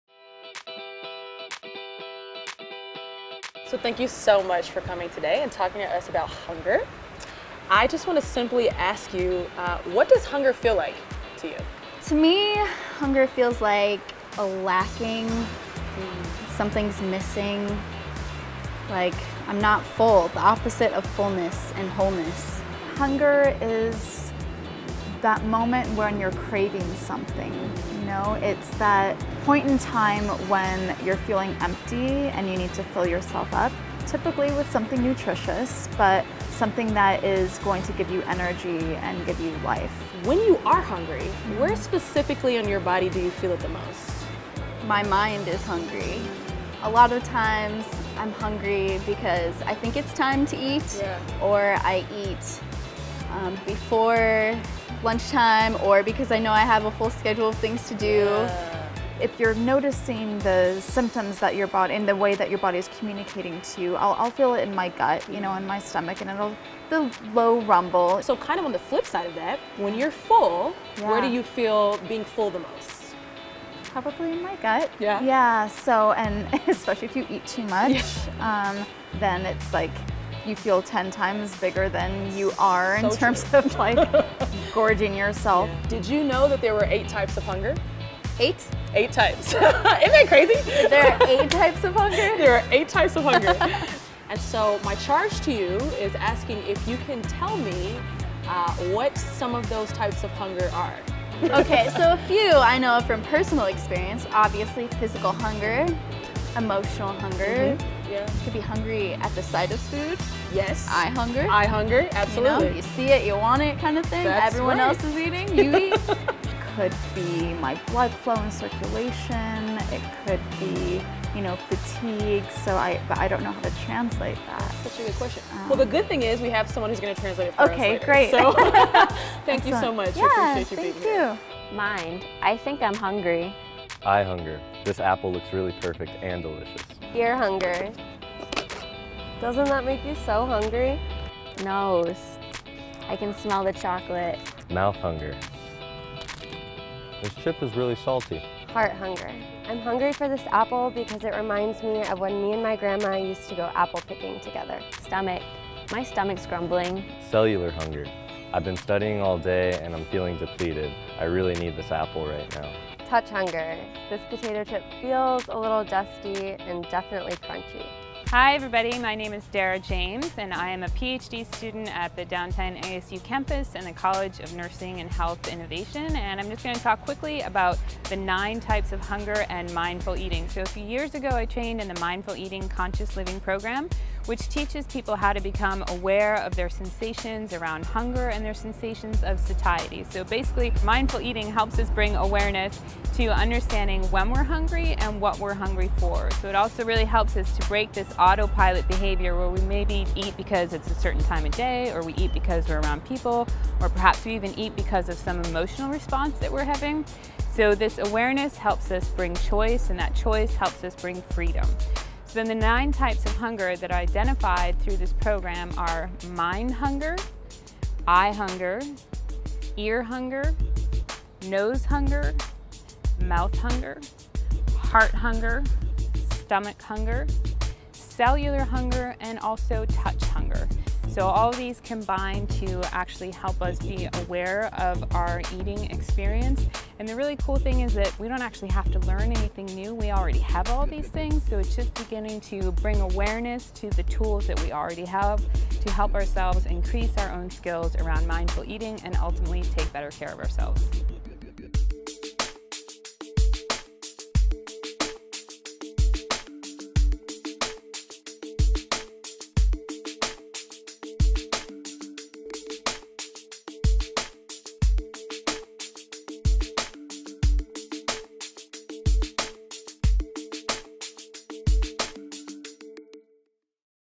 What does hunger feel like to you?
Did you know there are NINE types of hunger? Watch this video to explore how we feel hunger and what the 9 types of hunger are.